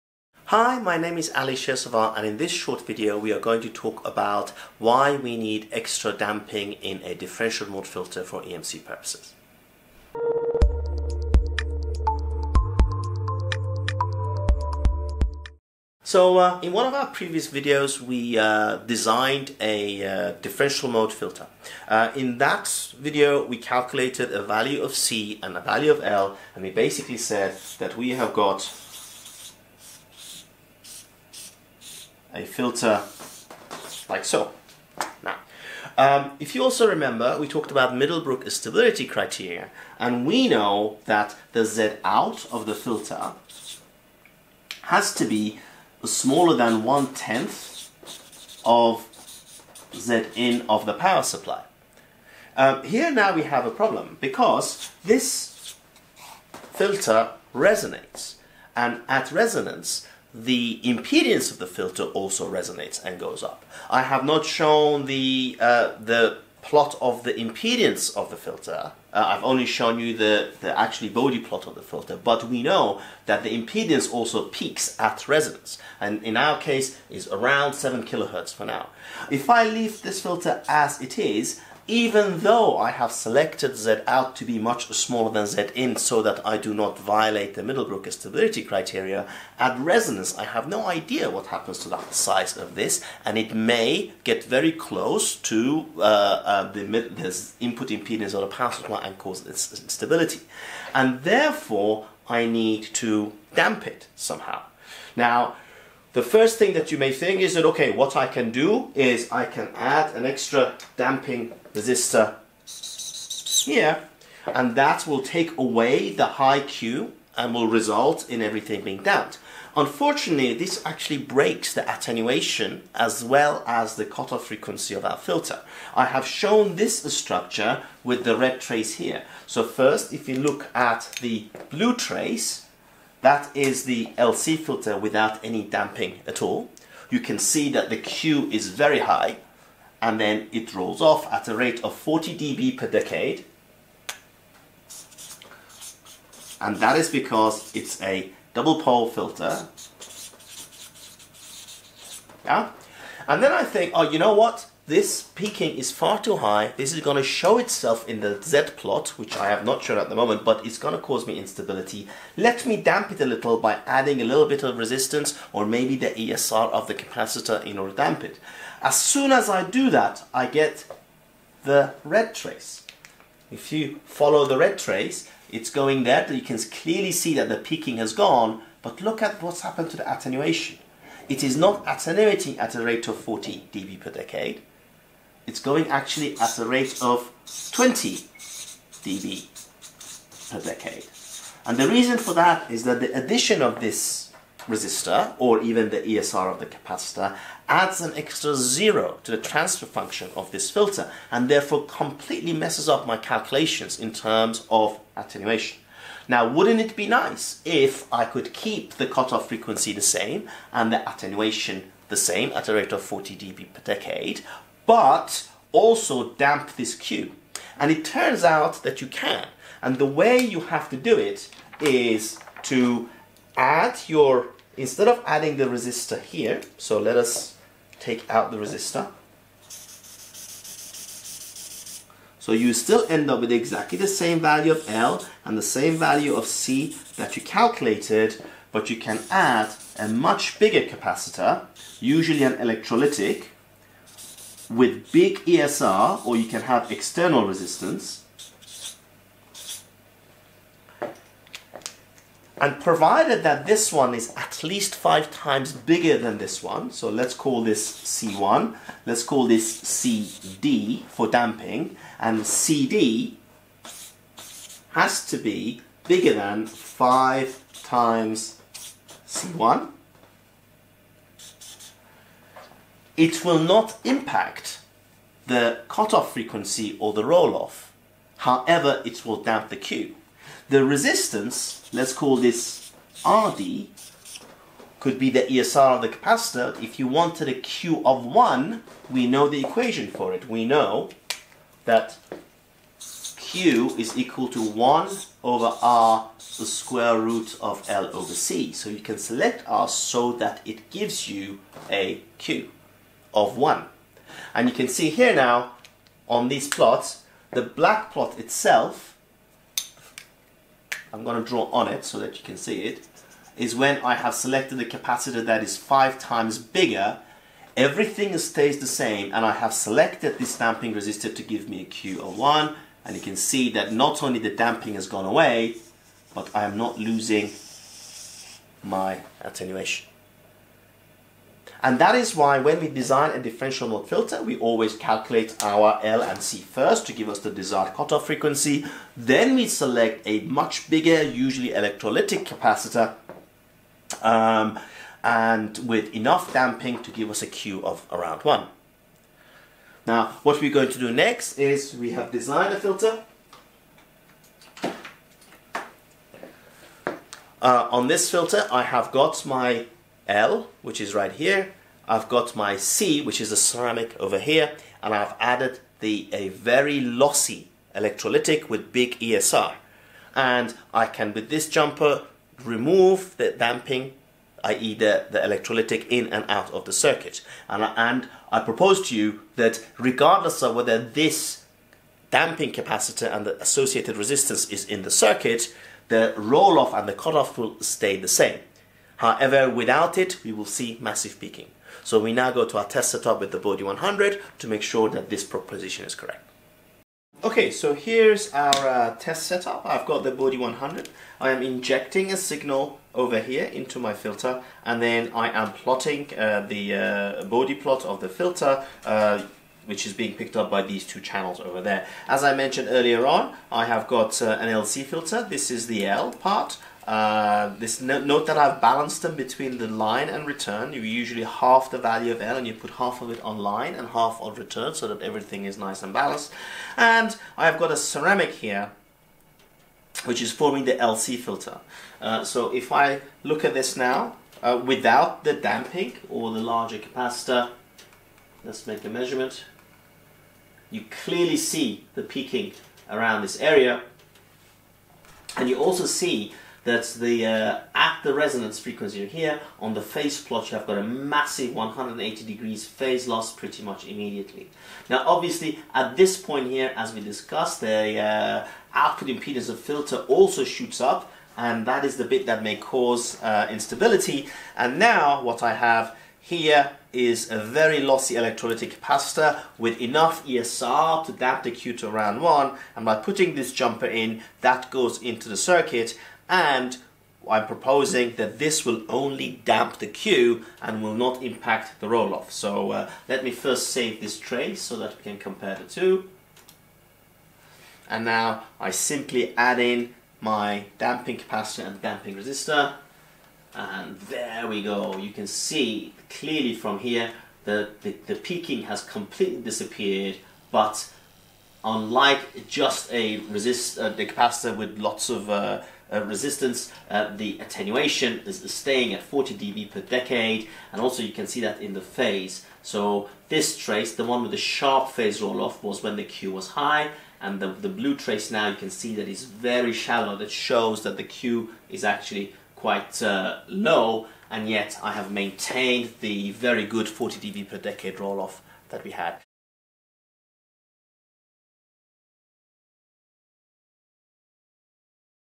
EMC Filter Design Part 5: Differential Mode Filter Damping Component Selection
In this video Dr Ali Shirsavar explains how to select your EMC input filter's damping components in such a way that they do not impair filter performance while meeting the Middlebrook's filter stability criteria. For further information on Biricha's EMC filter design workshop please